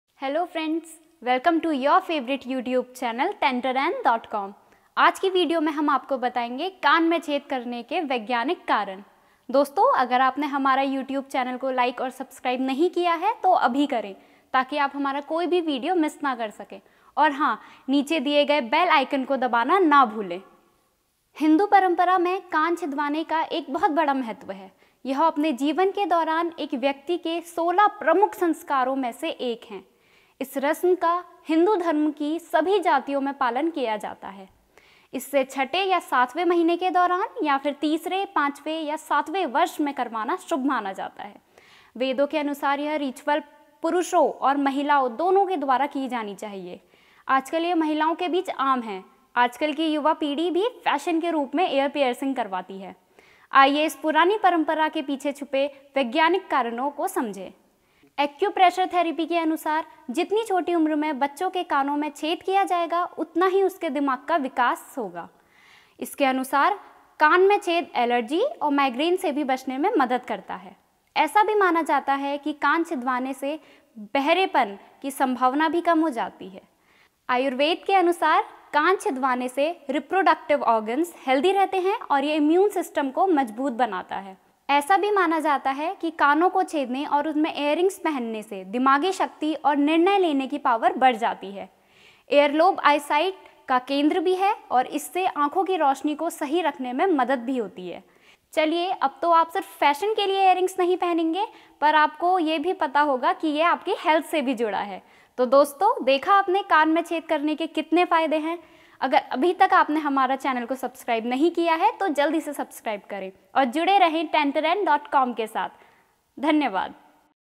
हिंदू धर्म में क्यों जरूरी है कान छिदवाना | Amazing Scientific Reasons Behind Hindu Traditions
Amazing Scientific Reasons Behind Hindu Traditions

हिन्दू परंपरा में कान छिदवाने का एक बहुत बड़ा महत्व है।  यह अपने जीवन के दौरान एक व्यक्ति के सोलह प्रमुख संस्कारों में से एक है। इस रस्म का  हिंदू धर्म की सभी जातियों में पालन किया जाता है। यह छठे या सातवें महीने के दौरान या फिर तीसरे,पांचवे या सातवें वर्ष में करवाना शुभ माना जाता है। वेदों के अनुसार,यह रिचुअल पुरुषों और महिलाओं दोनों द्वारा किया जाना चाहिए।